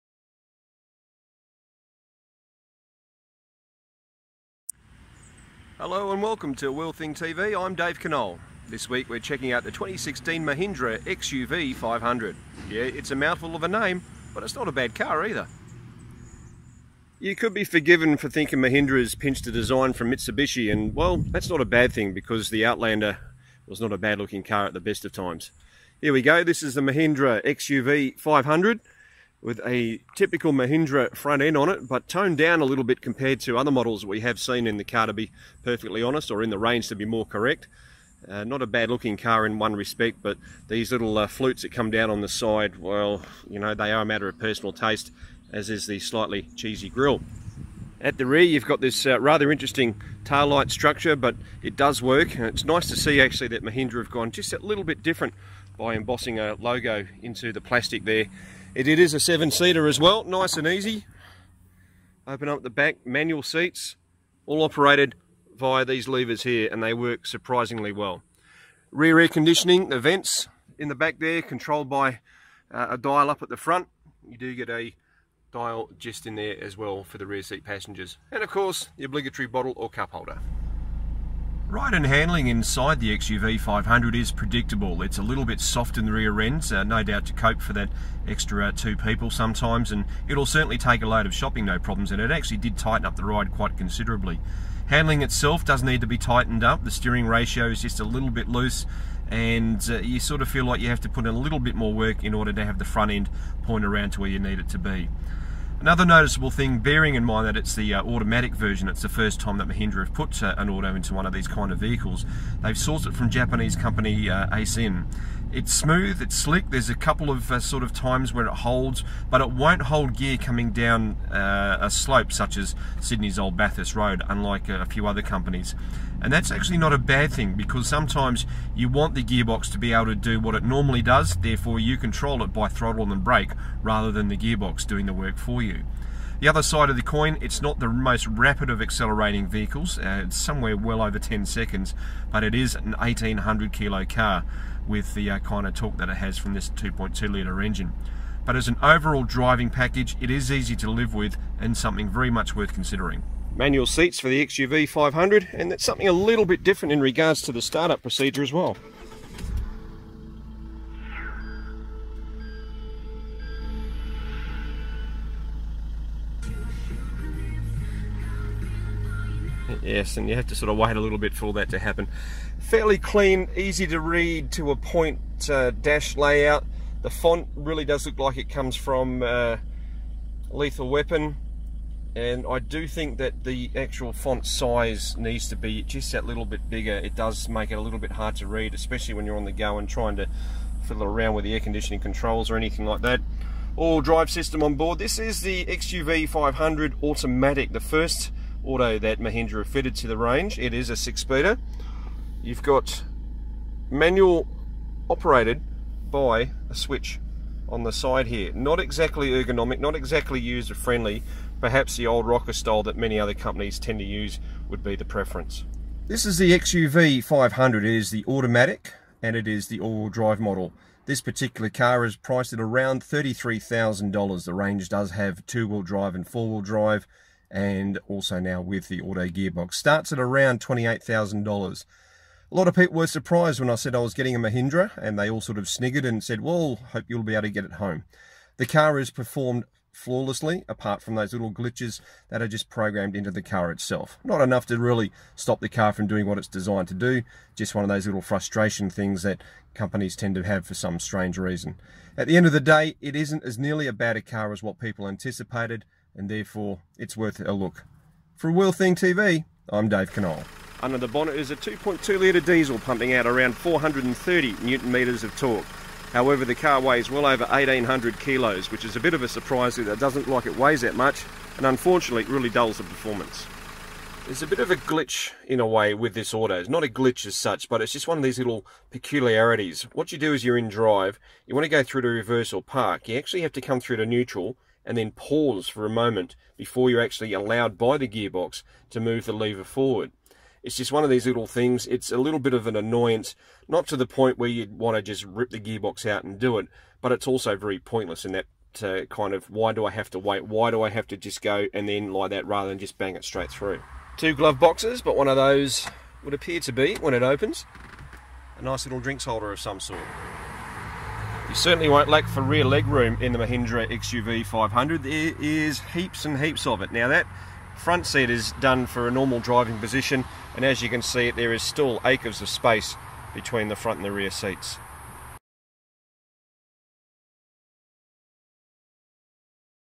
2017 Mahindra XUV500 diesel auto: A Wheel Thing TV review.
Put away your prejudice, it's better than that.